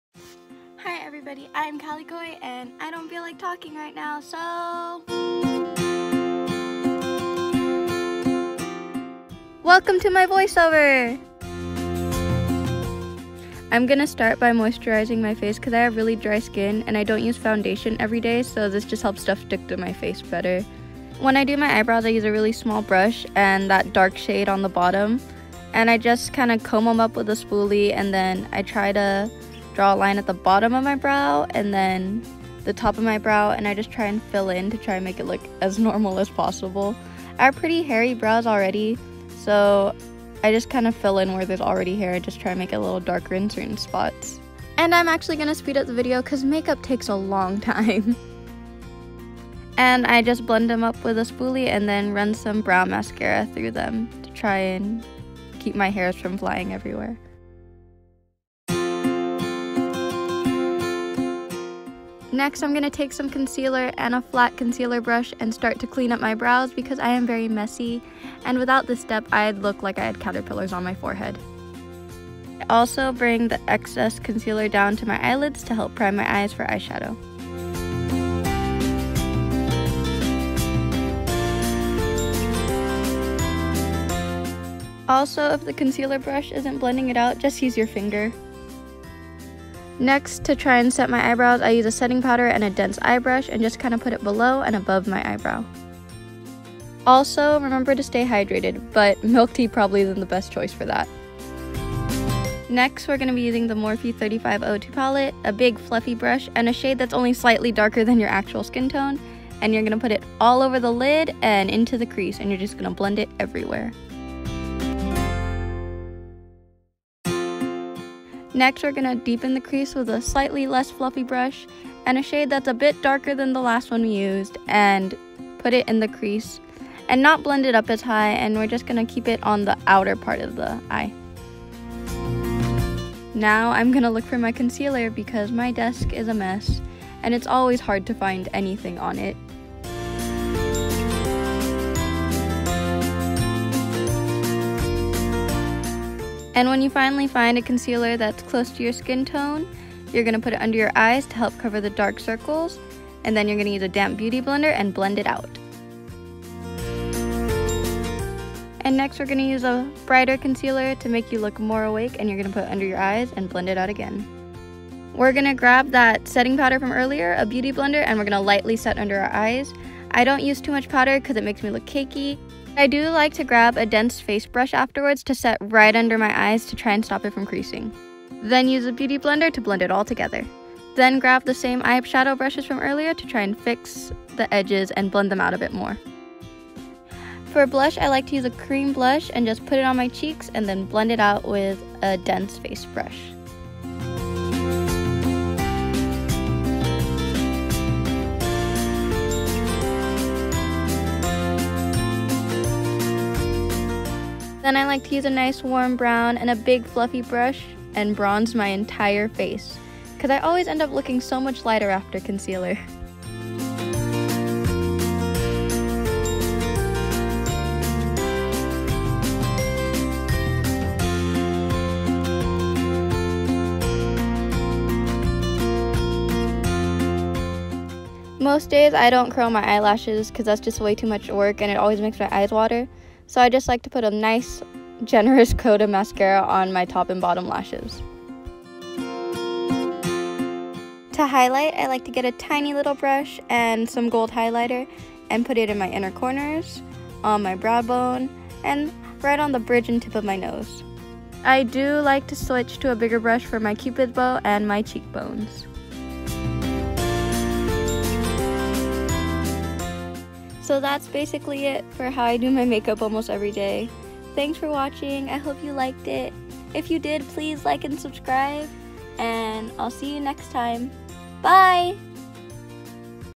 EVERYDAY MAKEUP | calikoi
this is how i do my makeup when i’m not being extra. twitter